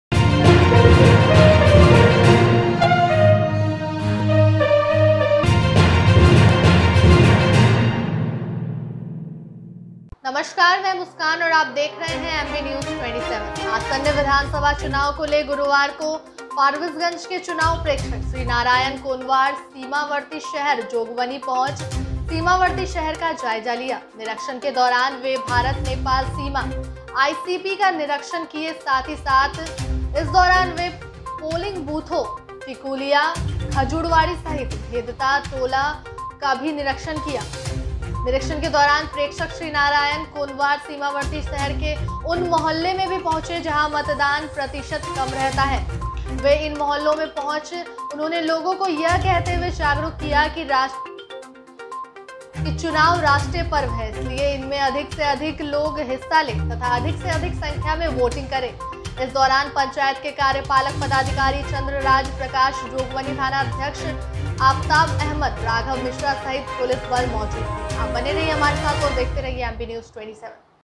फारबिसगंज के चुनाव प्रेक्षक श्रीनारायण कोनवार सीमावर्ती शहर जोगबनी पहुँच सीमावर्ती शहर का जायजा लिया
जोगबनी,
                         आसन्न विधानसभा चुनाव को ले गुरुवार को फारबिसगंज के चुनाव प्रेक्षक श्री नारायण कोनवार सीमावर्ती शहर जोगबनी पहुँच सीमावर्ती शहर का जायजा लिया. निरीक्षण के दौरान वे भारत नेपाल सीमा, आईसीपी का निरीक्षण किये साथ ही इस दौरान वे पोलिंग बूथों टिकुलिया, खजूरवाड़ी सहित भेदता टोलों का भी निरीक्षण किये. निरीक्षण के दौरान प्रेक्षक श्री नारायण कोनवार सीमावर्ती शहर के उन मोहल्ले भी पंहुचे जहां मतदान प्रतिशत कम रहता है. वे इन मोहल्लों में पहुच उन्होंने लोगो को यह कहते हुए जागरूक किया की चुनाव राष्ट्रीय पर्व है इसलिये इसमें अधिक से अधिक लोग हिस्सा ले तथा अधिक से अधिक संख्या में वोटिंग करें. इस दौरान उनके साथ जोगबनी नगर  पंचायत के कार्यपालक पदाधिकारी चंद्रराज प्रकाश, जोगबनी थानाध्यक्ष आफताब अहमद, राघव मिश्रा सहित पुलिस बल मौजूद थे.